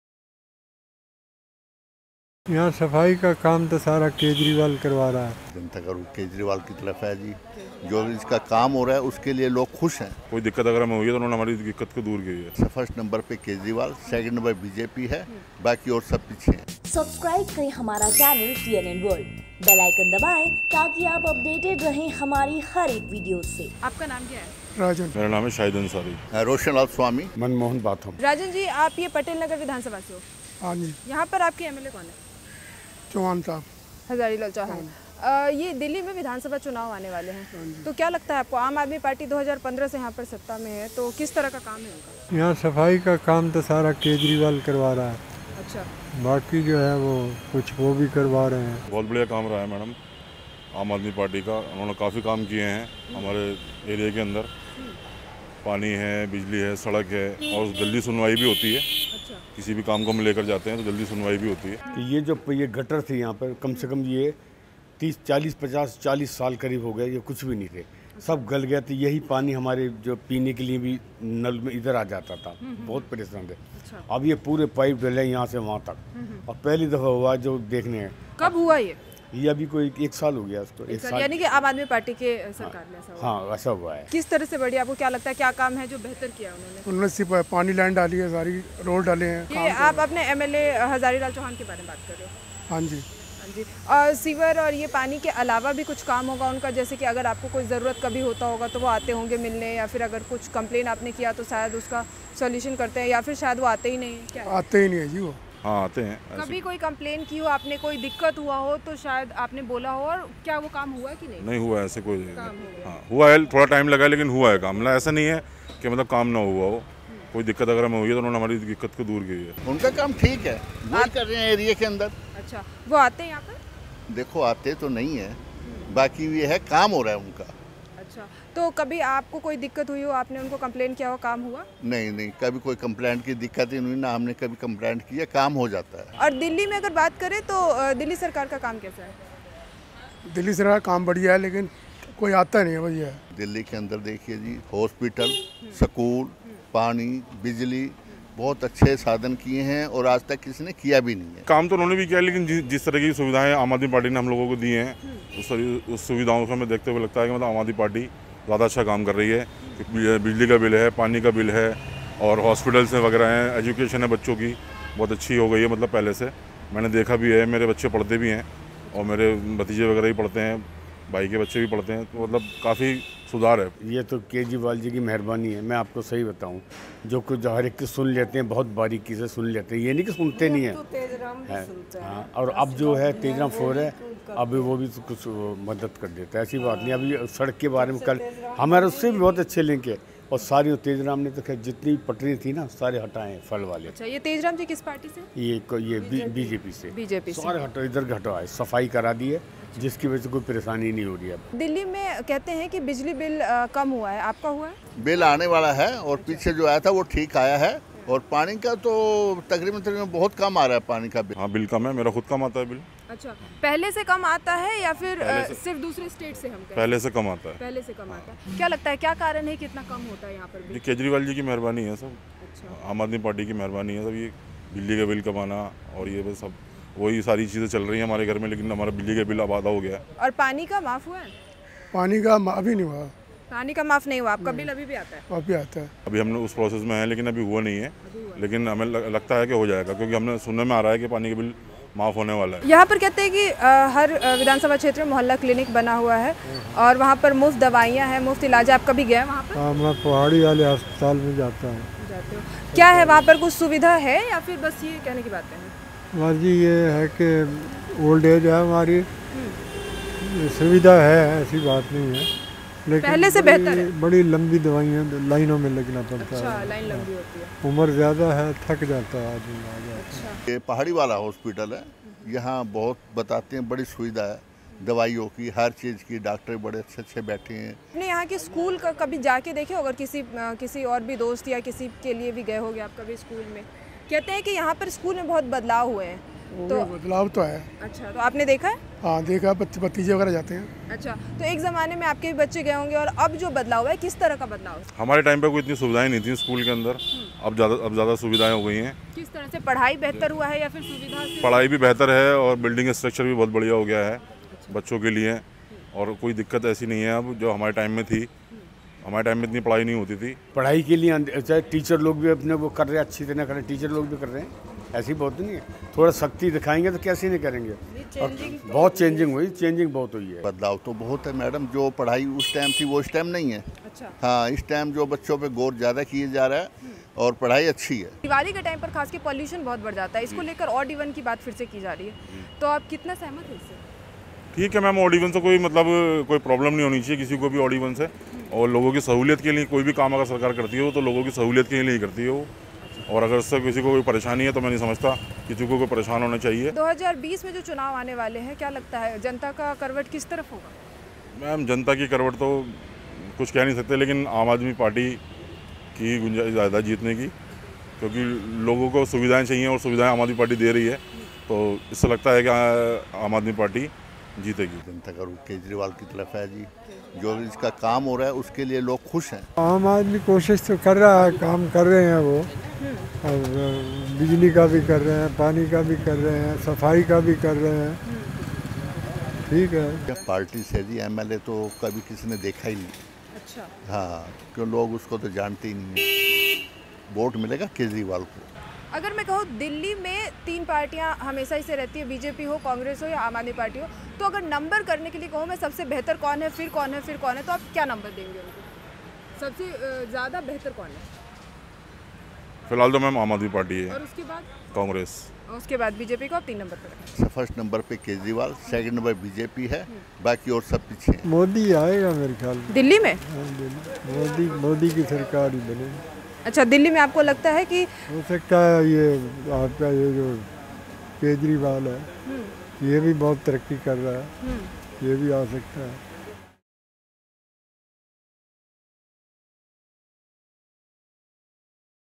Delhi Elections Public Opinion | Patel Nagar Vidhan Sabha | TNN WORLD
Delhi Assembly Elections countdown has started and its merely 100 days left .
So we went to Delhi Patel Nagar Vidhan Sabha's public and asked if their votes are going to BJP or Congress or to AAM AADMI PARTY.

Must watch public reaction on delhi elections .
kejriwal Work in delhi .

Anchor- Minu Verma
Camera- Rajnish